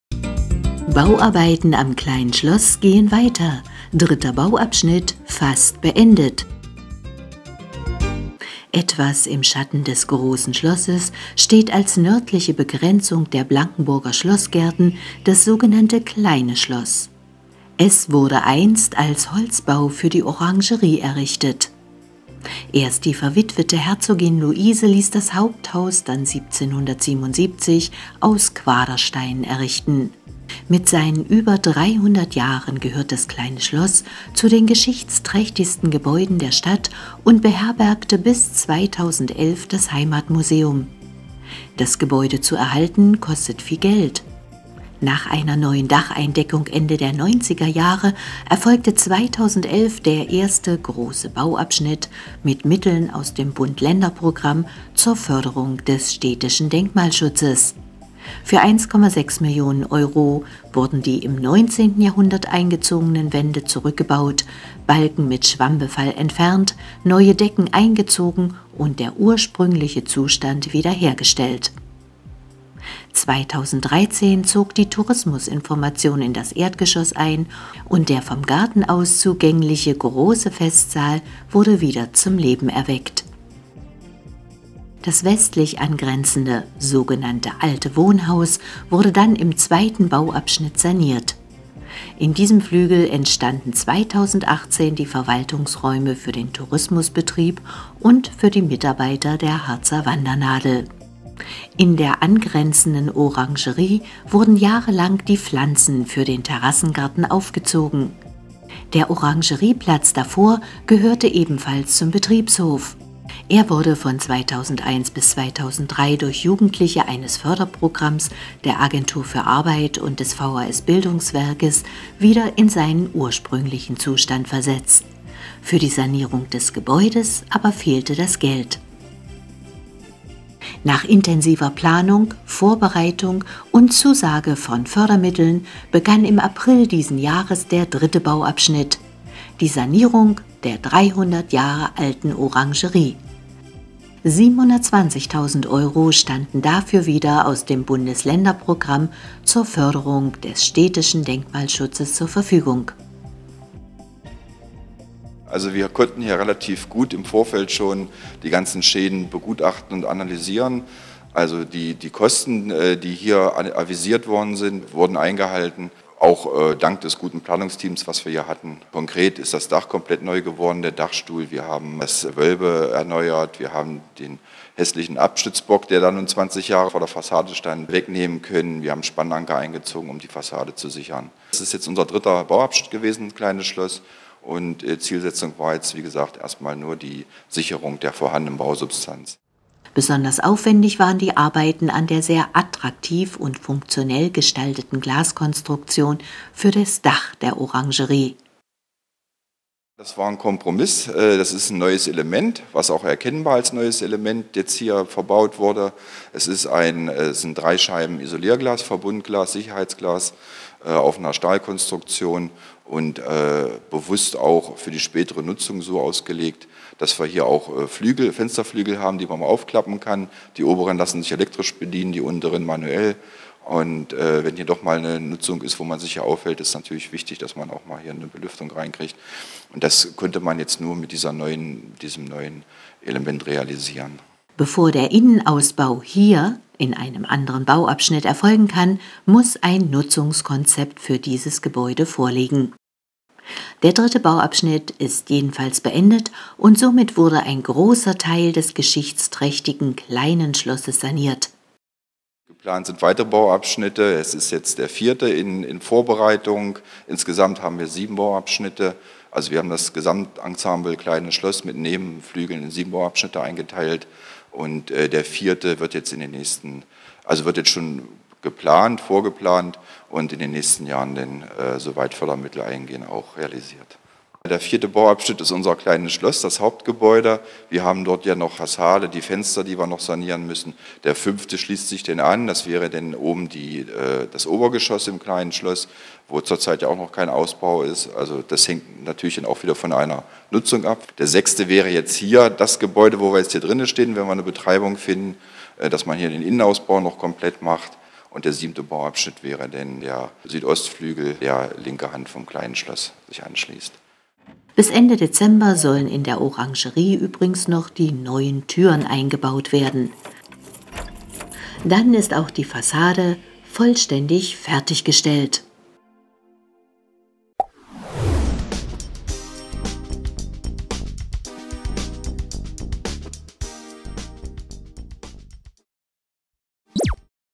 Bauarbeiten am Kleinen Schloss gehen voran - 3. Bauabschnitt fast beendet - RFH aktuell
►Thema:
Bauarbeiten am Kleinen Schloss gehen voran - 3. Bauabschnitt fast beendet